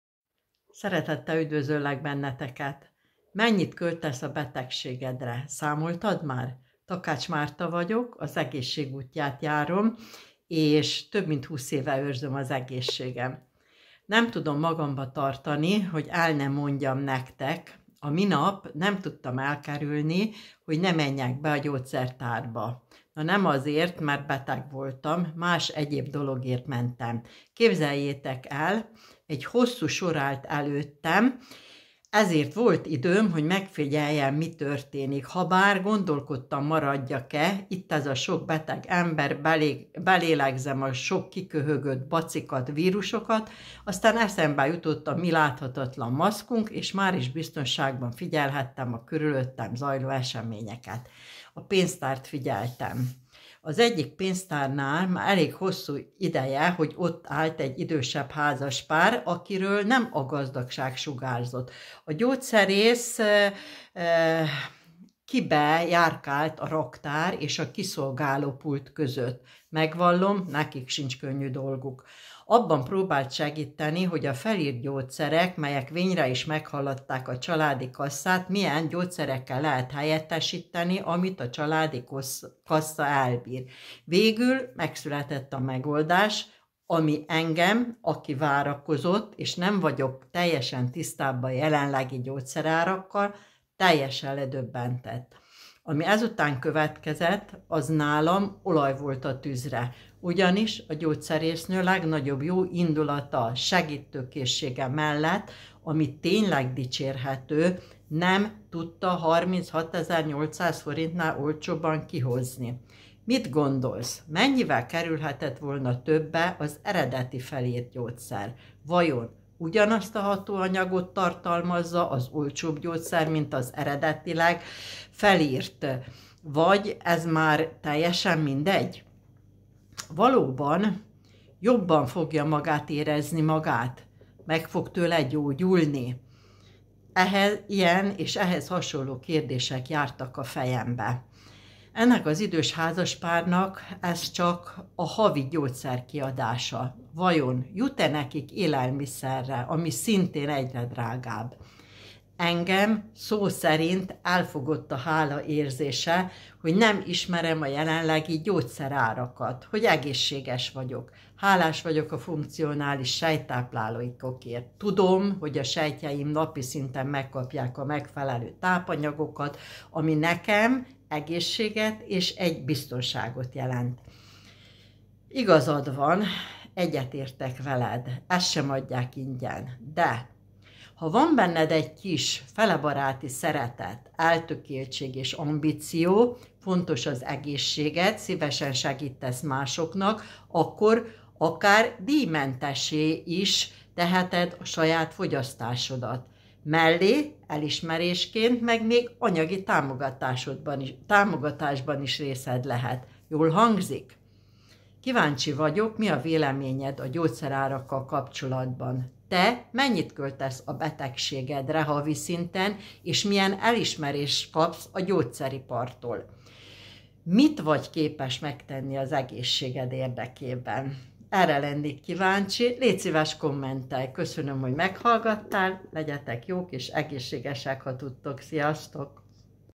Mennyit költesz a betegségedre?
Eleged van abból,  hogy egyre többet költesz a betegségedre?

Mennyire érzed jobban magadat a gyógyszerektől?

Mit szólnál ahhoz, ha tényleg egészségesnek erezhetned magad?